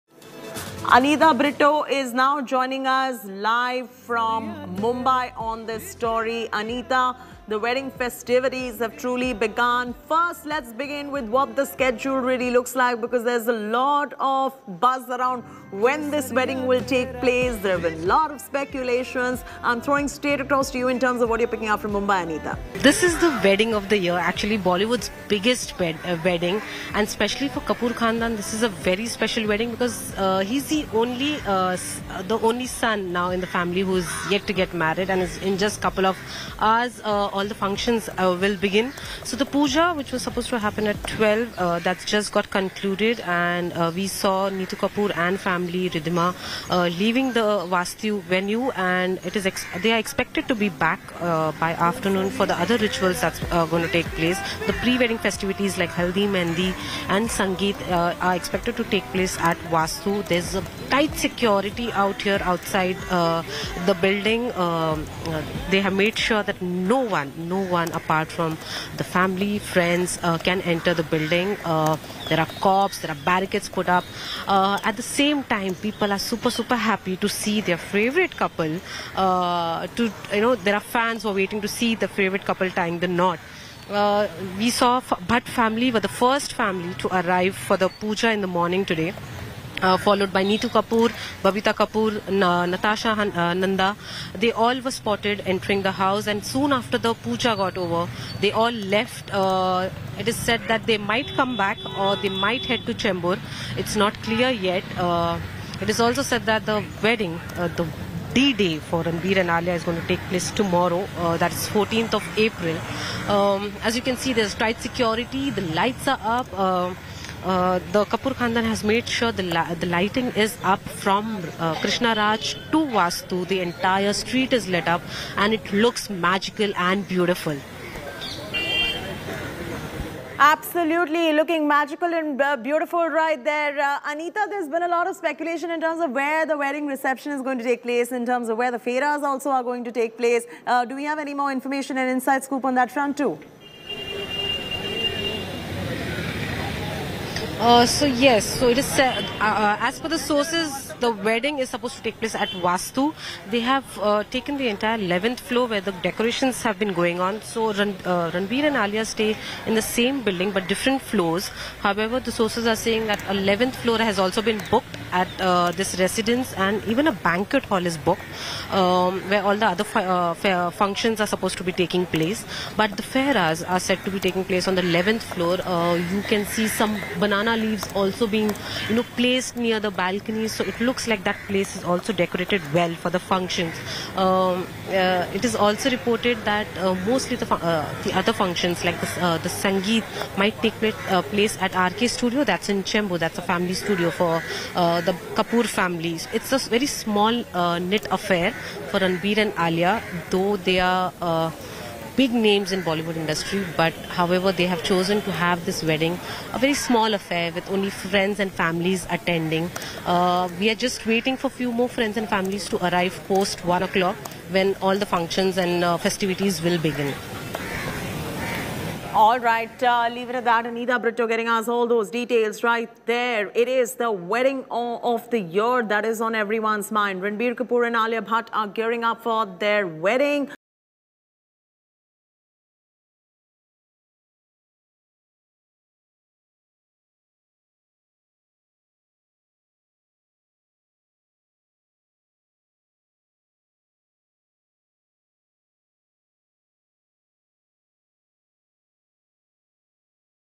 Alia Bhatt-Ranbir Kapoor Wedding LIVE Updates: Neetu, Riddhima Leave For Chembur
Alia Bhatt and Ranbir Kapoor's grand wedding is just around the corner. The couple's pre-wedding functions begin today, April 13, and we cannot wait to see the stunning pictures. RaAlia (Ranbir+Alia), as their fans fondly call them, have been in a relationship for around five years now. Here we will get you all the latest updates about Ranbir and Alia's private wedding, which will take place at RK House in Chembur, Mumbai.